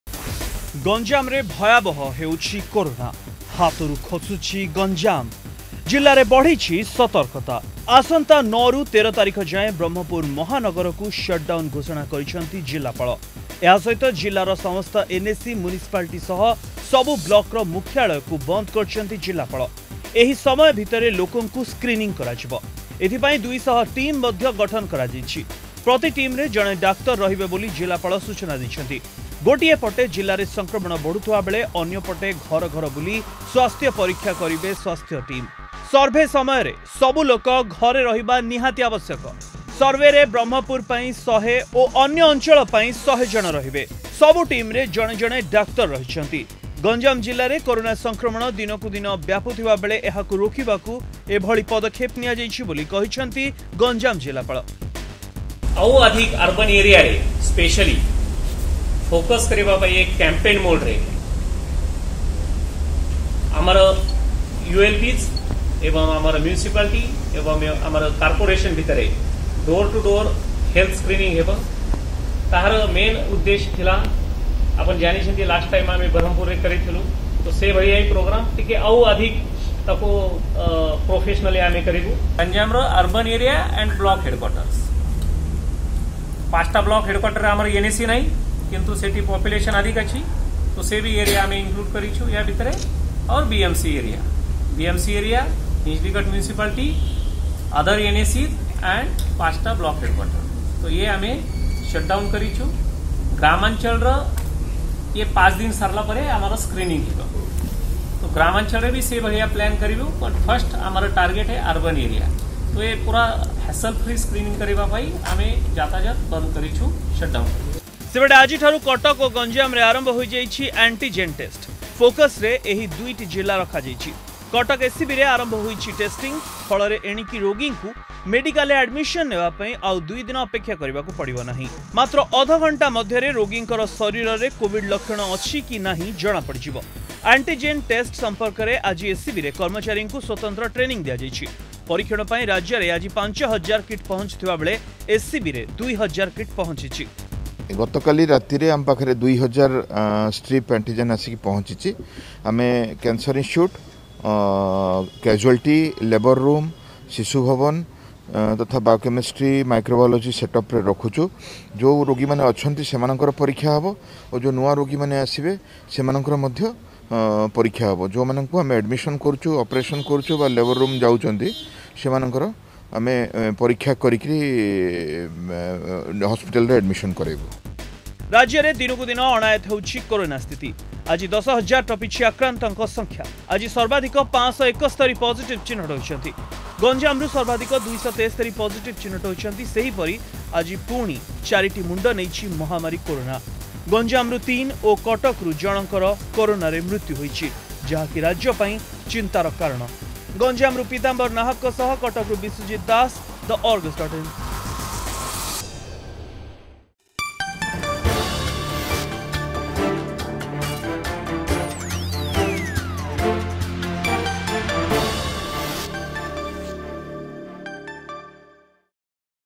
ଗଞ୍ଜାମରେ ଭୟାବହ ହେଉଛି କରୋନା ...
ଗଞ୍ଜାମରେ ଭୟାବହ ହେଉଛି କରୋନା । ହାତରୁ ଖସୁଛି ଗଞ୍ଜାମ । ଜିଲ୍ଲାରେ ବଢିଲା ସତର୍କତା । ଆସନ୍ତା ୯ରୁ ୧୩ ତାରିଖ ଯାଏଁ  ବ୍ରହ୍ମପୁର ମହାନଗରକୁ ସଟଡାଉନ ଘୋଷଣା କଲେ ଜିଲ୍ଲାପାଳ